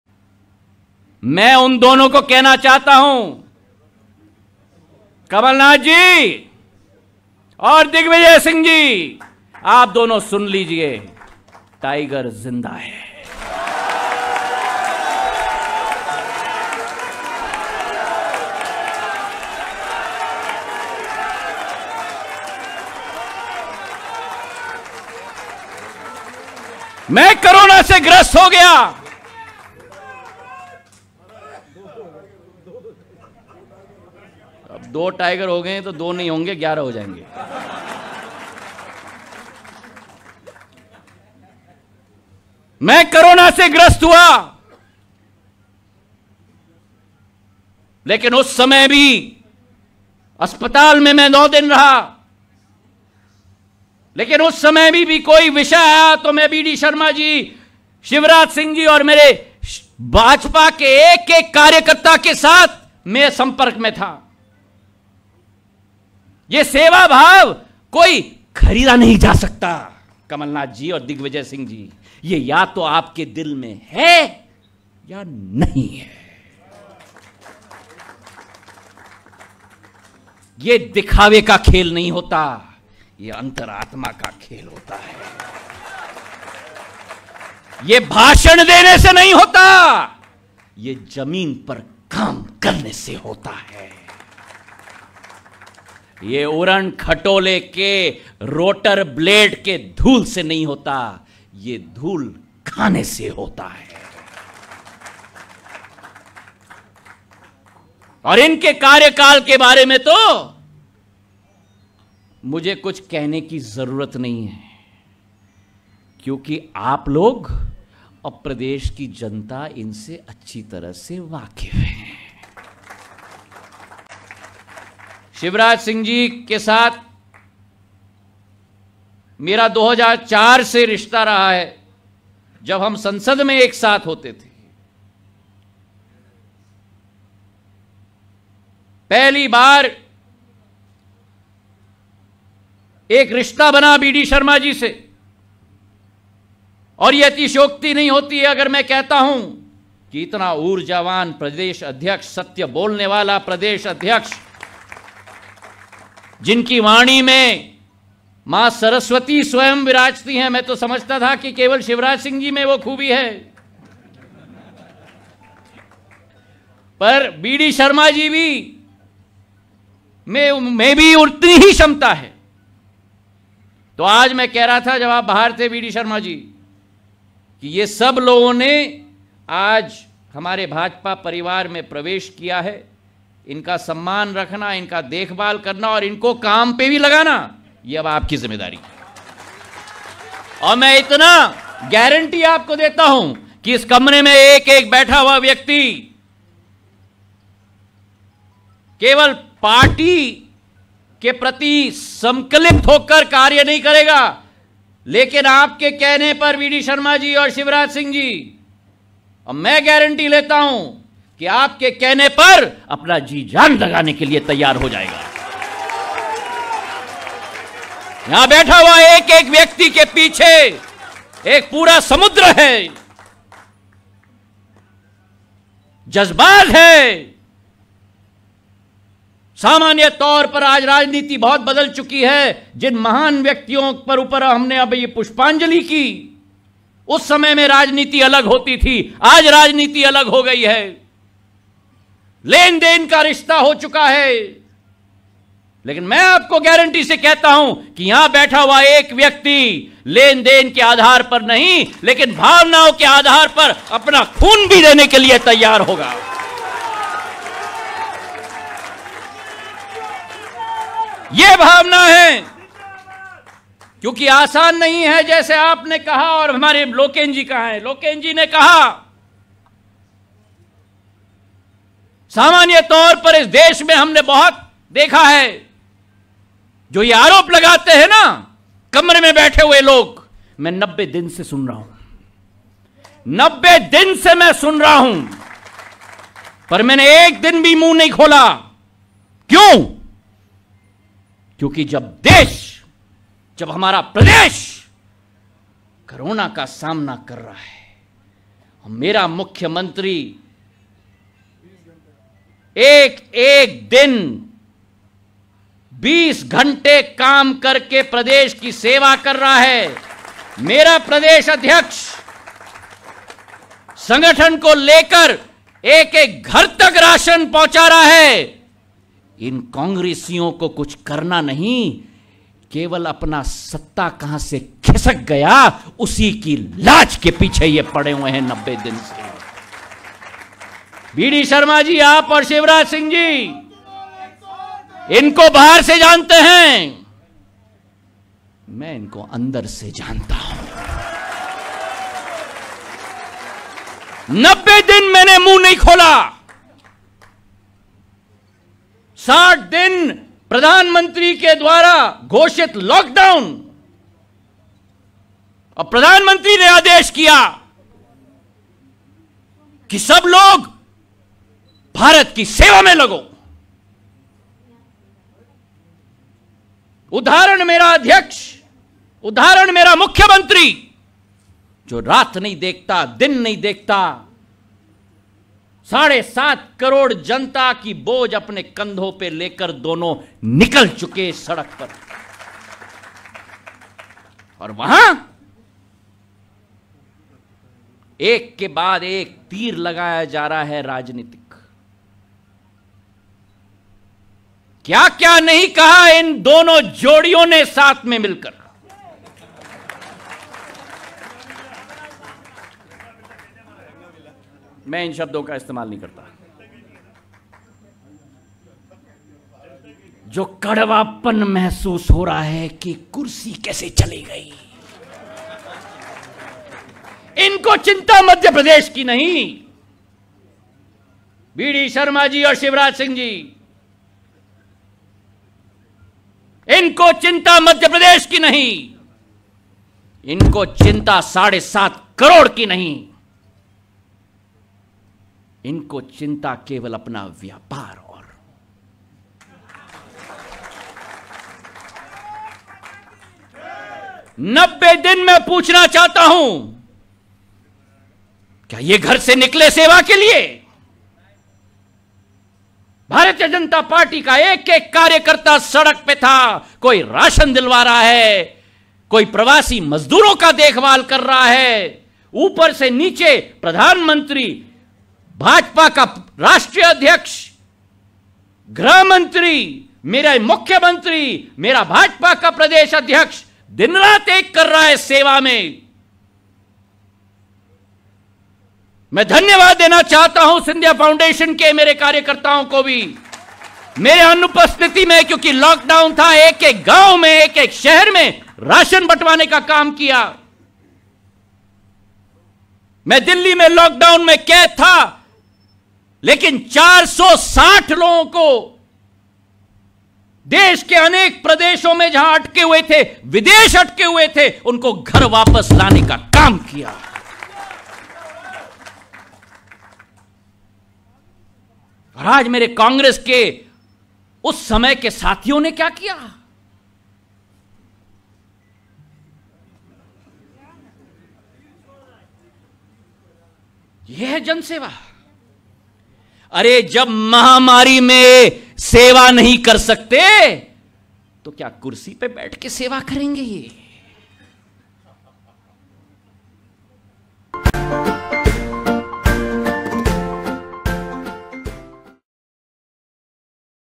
सिंधिया महाराज का कांग्रेस पर बड़ा हमला ! सुनिए क्या बोले LIVE
Jyotiraditya Scindia Speech on Digvijaya Singh & Kamal Nath From BJP Office Bhopal